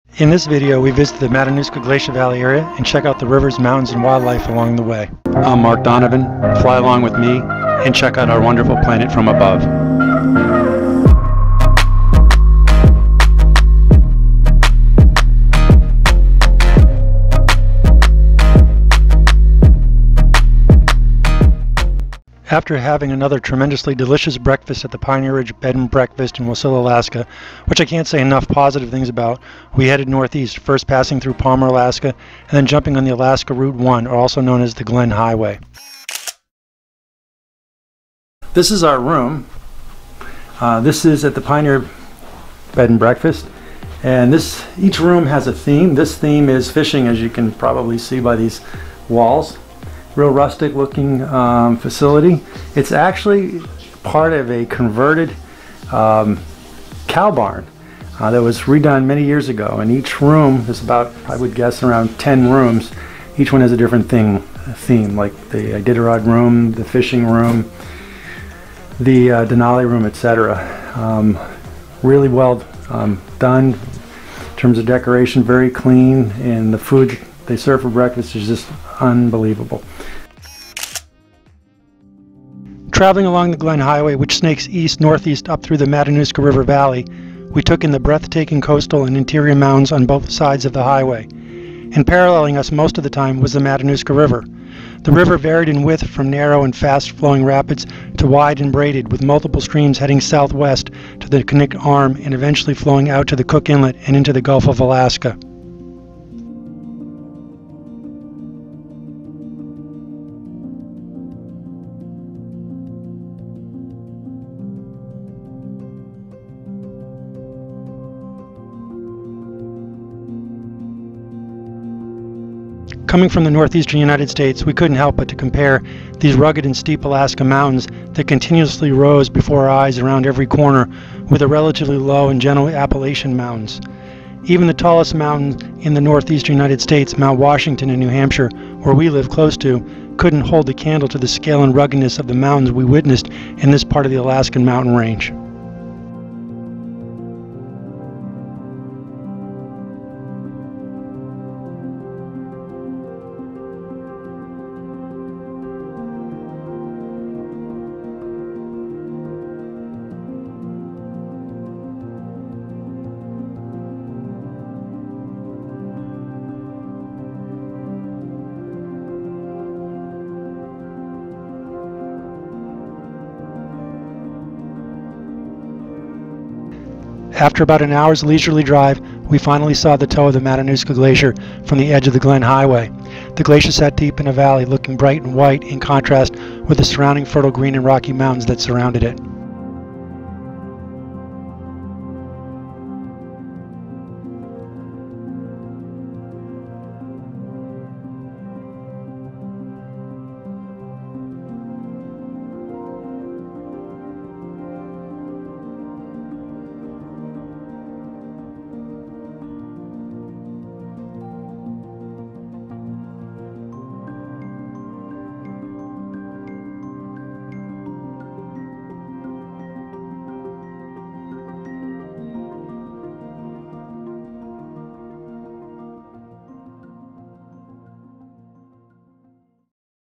Matanuska Glacier Glenn Highway Adventure
In this video we travel from Wasilla, Alaska, up through Palmer, Alaska, and then northeastwards up the Alaska Route 1, or Glenn Highway, to the Matanuska Glacier.  Along the way to the Matanuska Glacier, along the Glenn Highway, we paralleled the Matanuska River.